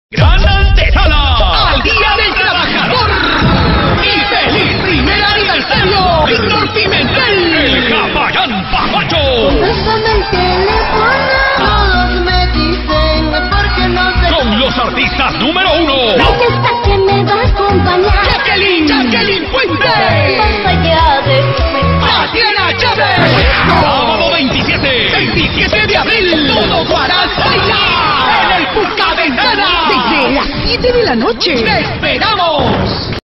GRAN ANIVERSARIO DE VICTOR PIMENTEL (JAPALLAN PAPACHO) HUARAZ 2013
Feliz I aniversario Victor Pimentel
Sábado 27 de abril
Local: pukaventana